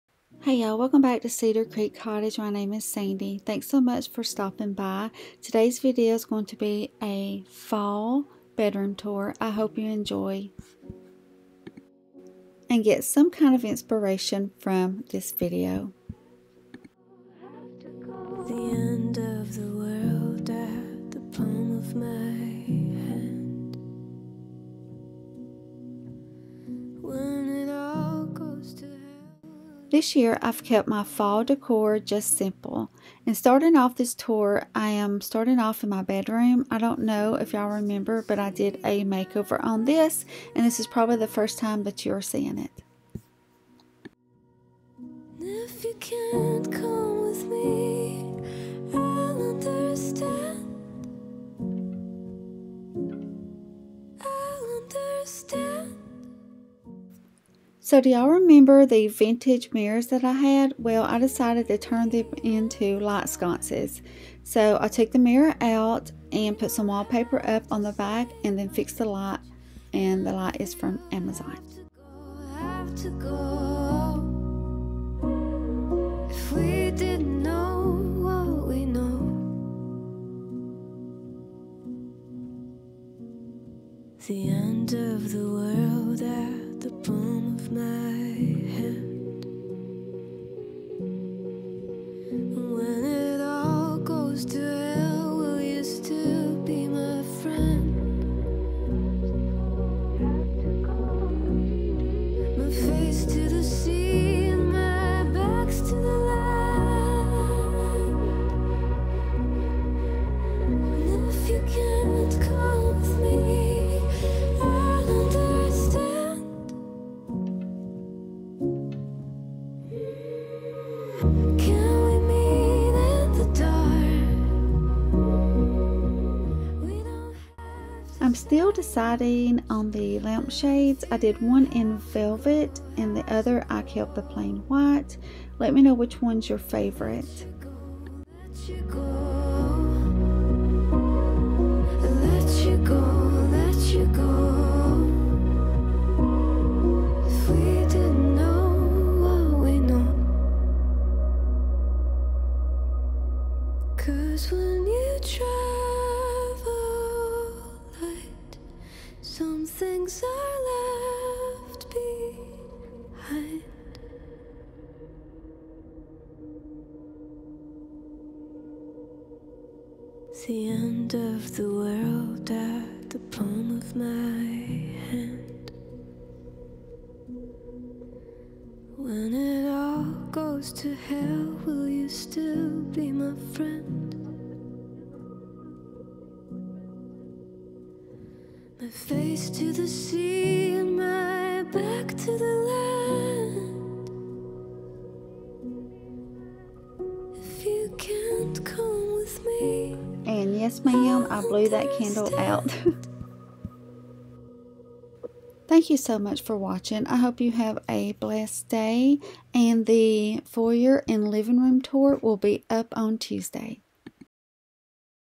MOODY FALL FRENCH COUNTRY BEDROOM TOUR 2024!
Hey Y'all! I hope you get some inspiration from todays video!! Please let me know if you need any information on any decor item!! HAPPY FALL !!!

XOXO ~SANDY

MUSIC~ ARTLIST
INSTAGRAM~CEDARCREEKCOTTAGE
TIKTOK~CEDARCREEKCOTTAGE